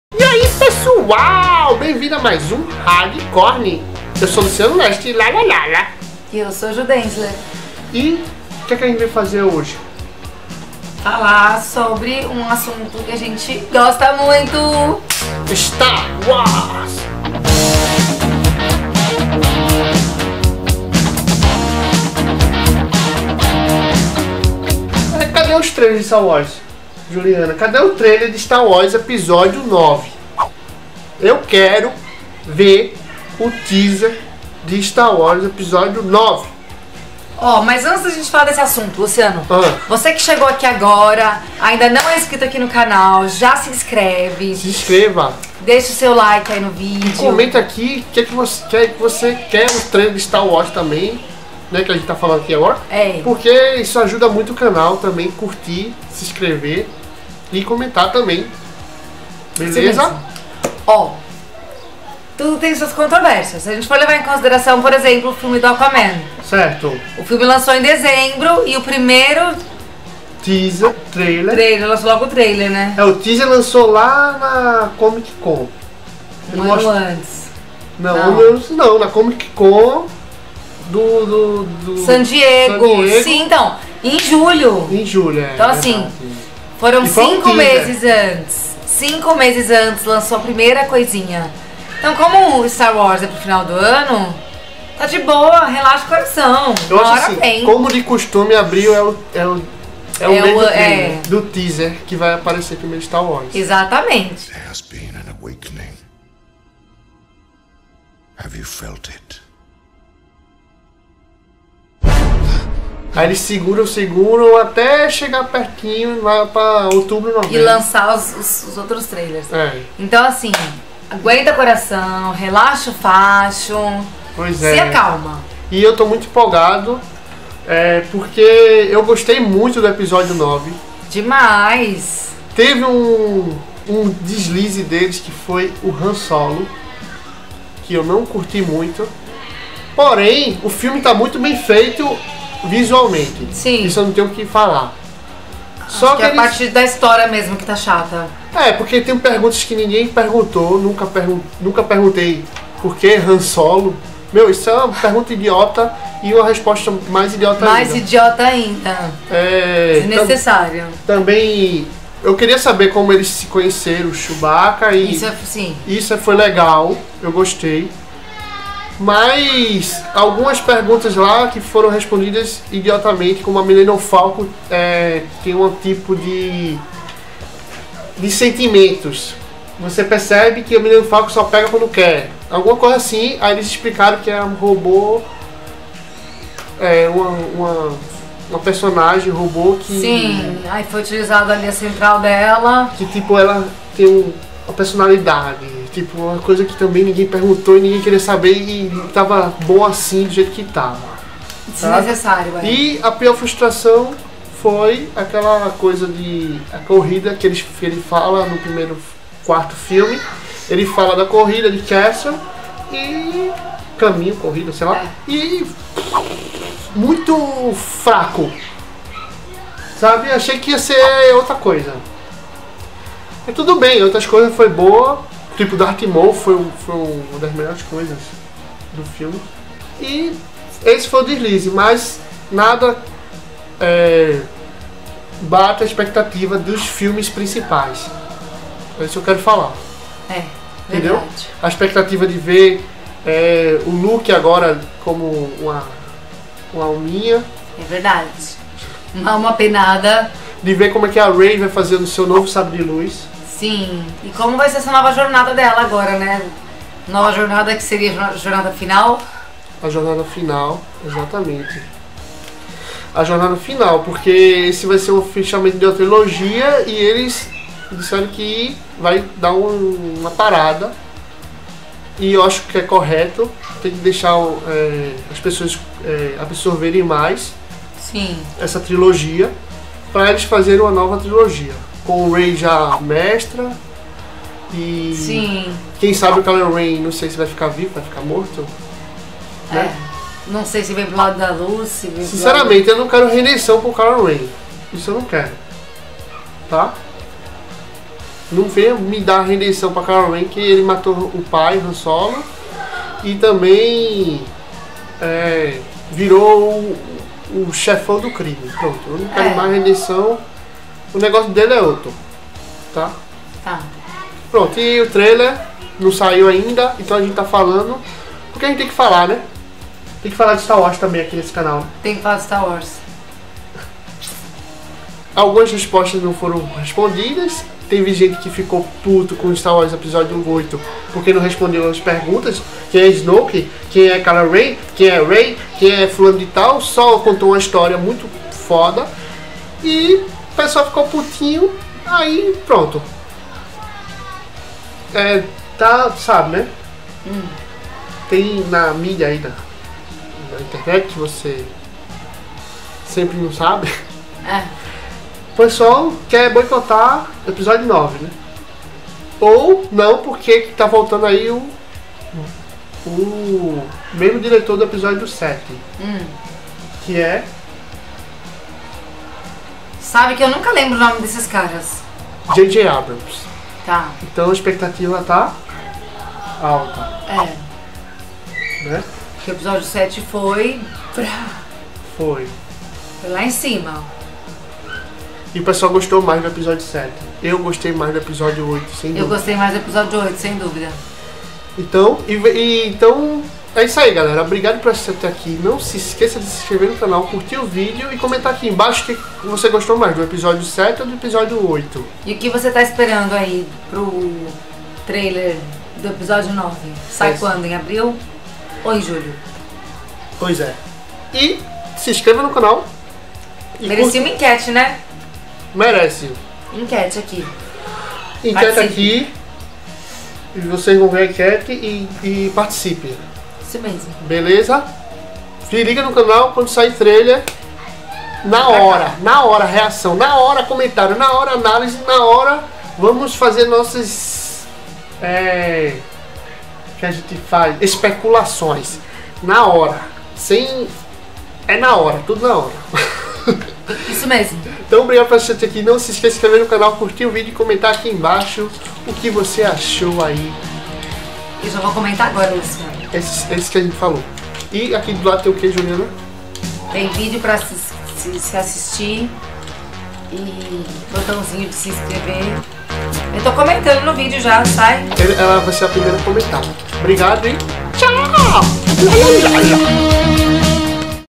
STAR WARS Episódio 9 estamos empolgados para ver logo o Teaser Trailer! | Precisamos Falar! HardCorn
Precisamos Falar sobre o quando vai sair o novo teaser trailer de Star Wars episódio 9, estamos empolgados pra fazer a reação [REACT] . Lucas Filme não nos deixe na mão!

Star Wars: Episódio IX é um futuro filme estadunidense do tipo space opera épico de aventura, fantasia e ficção científica de 2019, escrito por J. J. Wikipédia

Data de lançamento: 19 de dezembro de 2019 (Brasil)
Direção: J. J. Abrams
Séries de filmes: Terceira Trilogia Star Wars
Autor: George Lucas
Roteiro: J. J. Abrams, Chris Terrio

Parceiro (Jabba the Hutt):

Primeiro Império Galáctico Pernambuco oficial

Valeu!

Valeu!!
Viva o Rock!